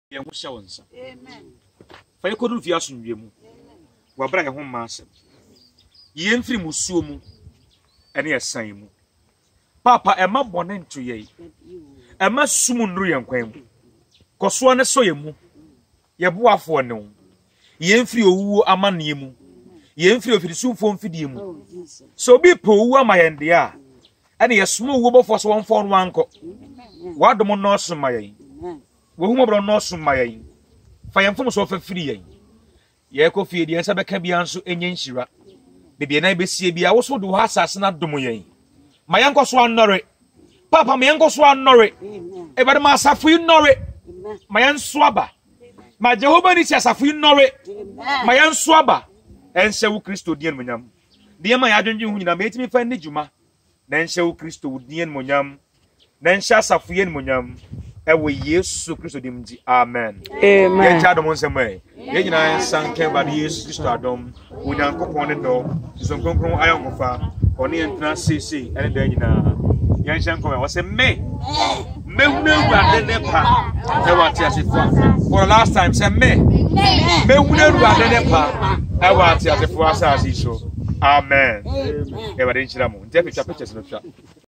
The the inspirational scripture to Jehovah Nissi Evangelical Ministry @ Adjamesu. Ksi. Psalms 27:13
I always prophesy this scripture Psalms 27: 13 after sharing the Benediction .

The joy of the Lord filled the church even though it has had a tough beginning .

unroofed , in the wilderness , though about a quarter kilometres from the center of the town

But the Prophetic word in Psalm 27:13 kept moving this little group of God"s great people

"For I remain Confident in this , that my eyes shall see the goodness of the Lord in the land of the living " Amen...